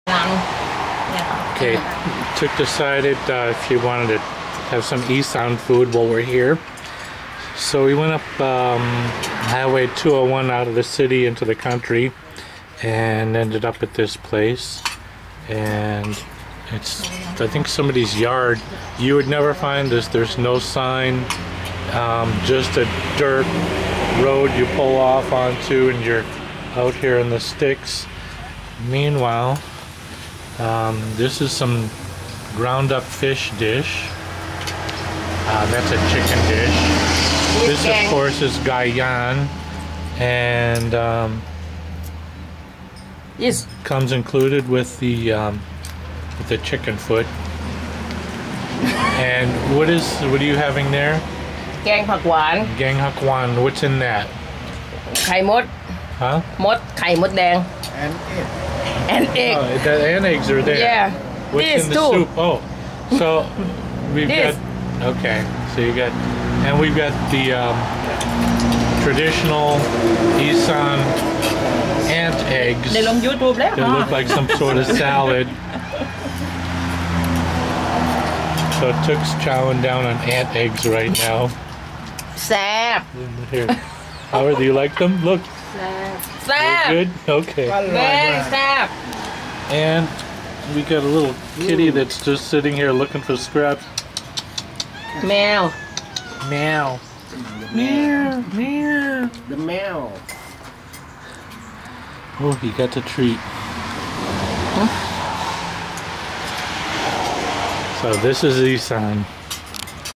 Ant Eggs, Ant Eggs, Roly Poly Ant Eggs
Tuk wanted some traditional Isaan Food.  (Probably the US equivalent to having dinner with Troy from Swamp People.)  Main dish...Ant Eggs.